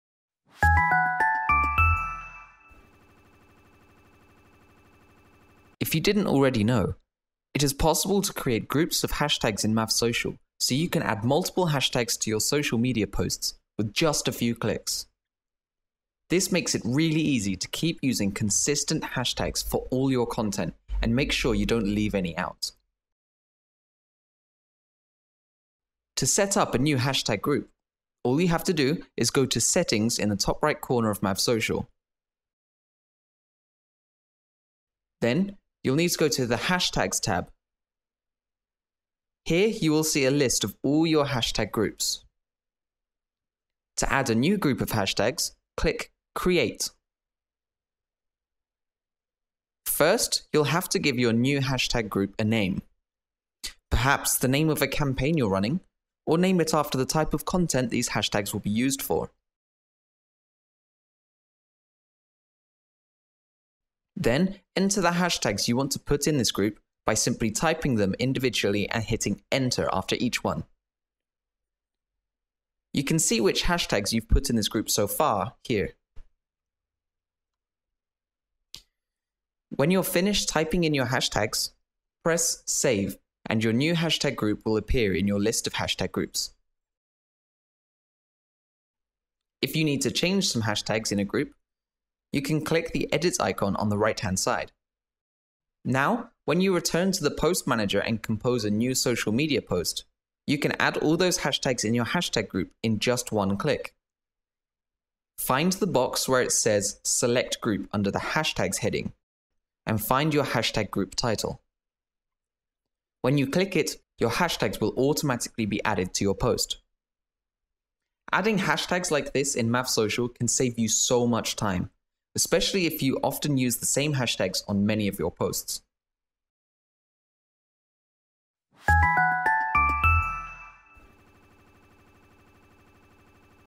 Creating Hashtag Lists (Add hashtags to posts in 1 click!) | MavSocial
MavSocial makes it easy to save lists of your favourite hashtags and add them to your social media posts in just a few clicks!

First, create customized Hashtag lists for you to add to your social media posts, then paste all those hashtags into your posts in the Post Manager by simply selecting the hashtag group.